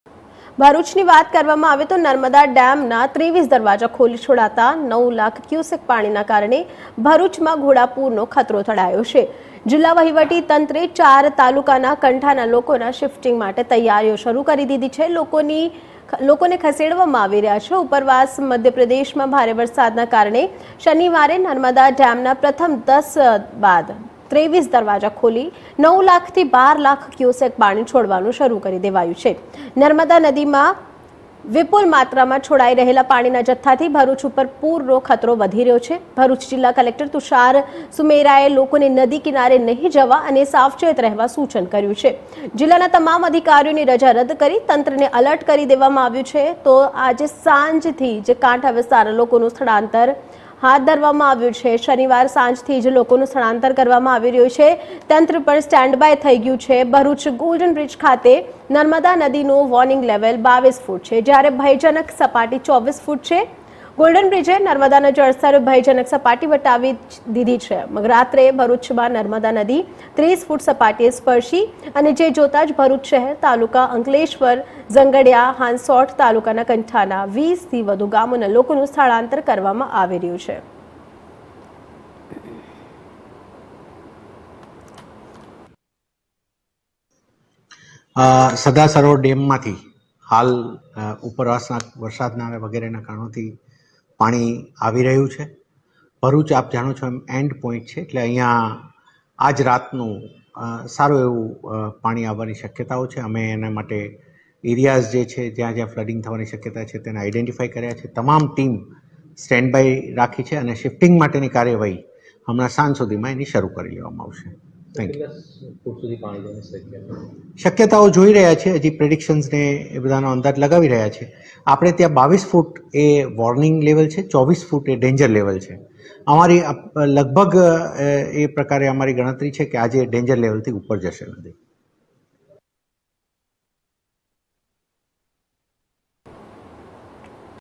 ભરૂચ પર ઘોડાપૂરનો ખતરો નર્મદા નદી 24 ફૂટની સપાટી વટાવી 30 ફૂટે પોહચવાની વકી - India News Gujarat
નર્મદા ડેમના 23 દરવાજા ખોલી છોડાતા 9 લાખ ક્યુસેક પાણીના કારણે ભરૂચમાં ઘોડાપુરનો ખતરો તોળાયો છે. જિલ્લા વહીવટી તંત્રે 4 તાલુકાના કાંઠાના લોકોના શિફ્ટિંગ માટે તૈયારીઓ શરૂ કરી દીધી છે. ઉપરવાસ મધ્યપ્રદેશમાં ભારે વરસાદના કારણે શનવારે નર્મદા ડેમના પ્રથમ 10 બાદ 23 દરવાજા ખોલી 9 લાખથી 12 લાખ ક્યુસેક પાણી છોડવાનું શરૂ કરી દેવાયું છે. નર્મદા નદીમાં વિપુલમાત્રામાં છોડાઈ રહેલા પાણીના જથ્થાથી ભરૂચ ઉપર પુરનો ખતરો વધી રહ્યો છે. ભરૂચ જિલ્લા કલેકટર તુષાર સુમેરાએ લોકોને નદી કિનારે નહિ જવા અને સાવચેત રહેવા સૂચન કર્યું છે.